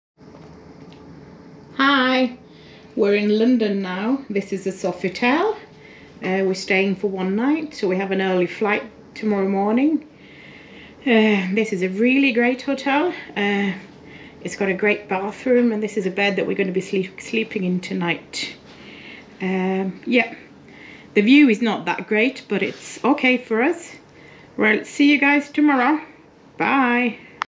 Sofitel Heathrow
Late night arrival into Heathrow and early connecting flight. Ok room for the purpose. Ground floor room with inside view. Quiet room though.